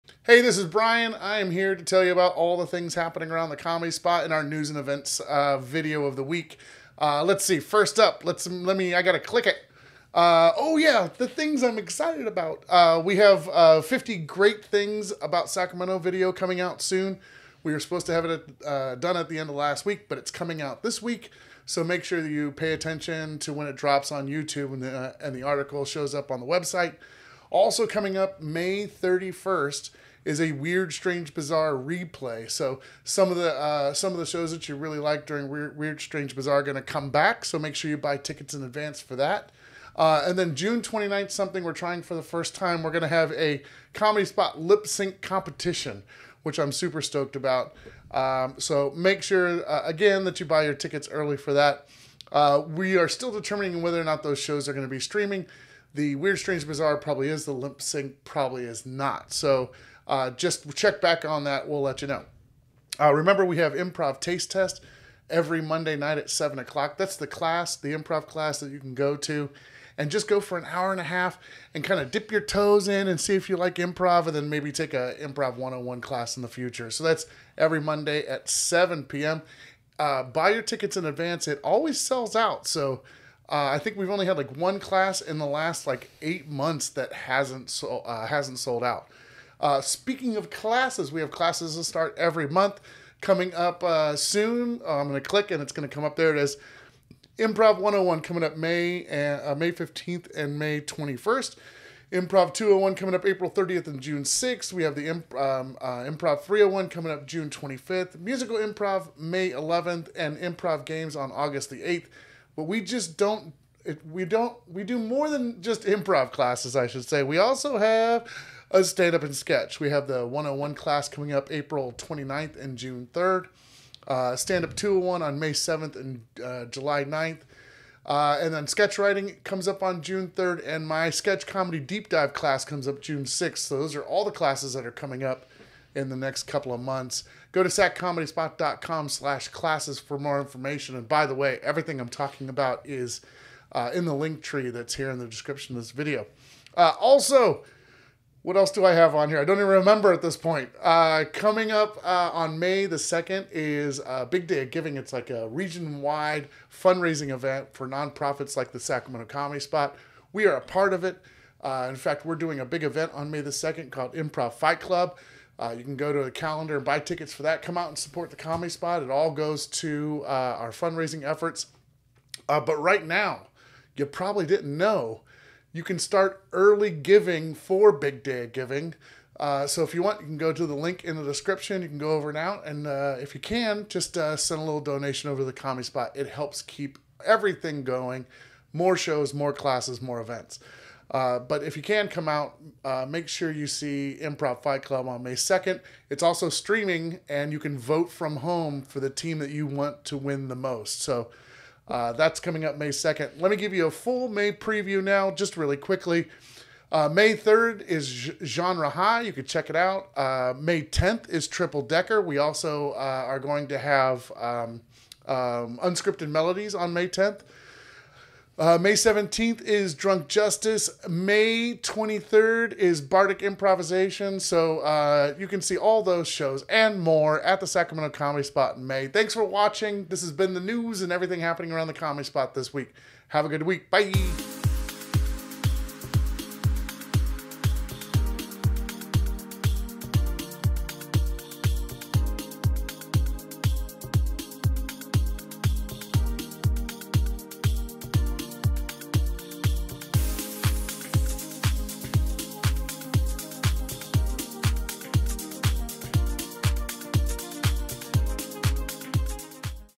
New and Events - April 23, 2024
Want to know what we are working on? Want to know what is coming up soon?  Watch these videos each week to be the first to know what is happening around the Comedy Spot.

Big Day of Giving Early Giving started April 18th.